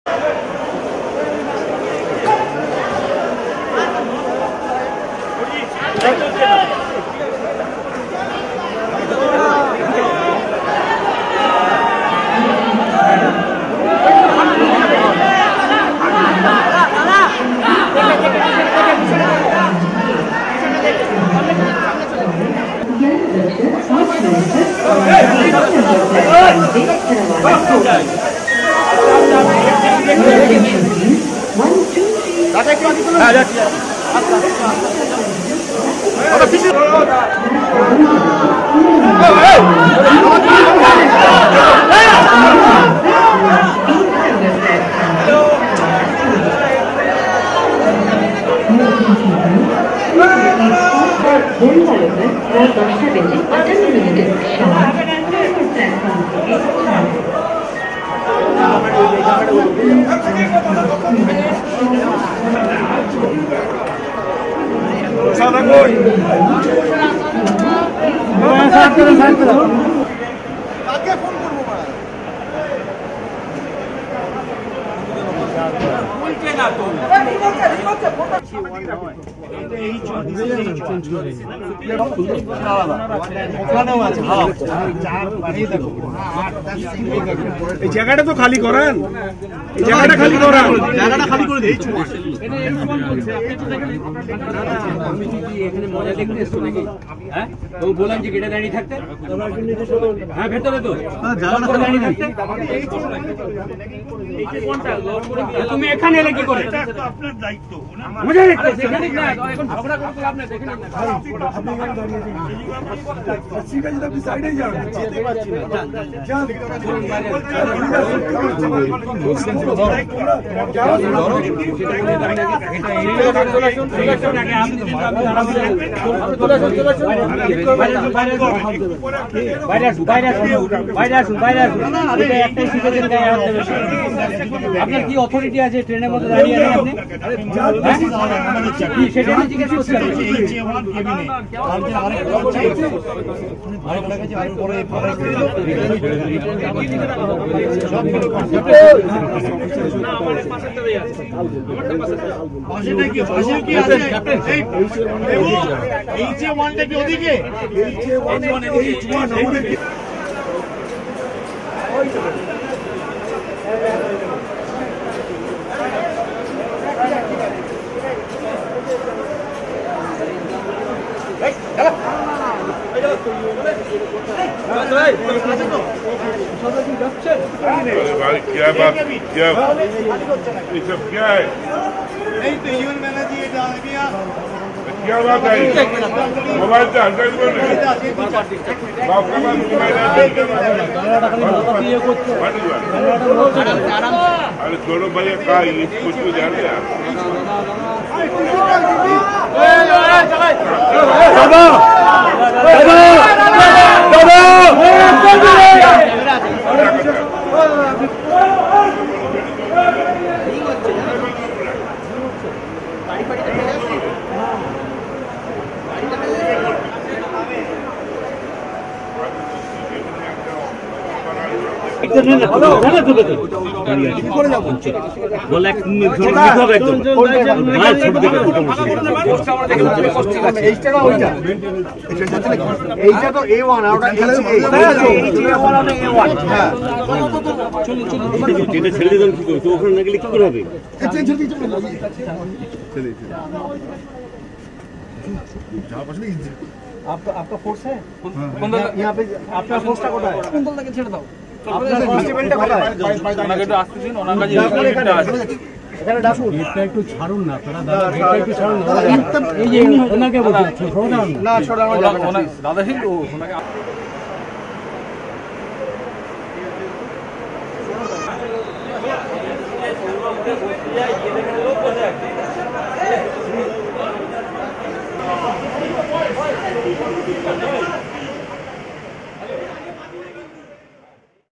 Saurav Ganguly Fight with passenger in the train while travelling
The former indian captain saurav ganguly had bit of brawl with a passenger while travelling in train. The matter was due to the seat allotment. Dada was forced to travel in Ac 2 tier as he booke for Ac 1. Watch the full incident.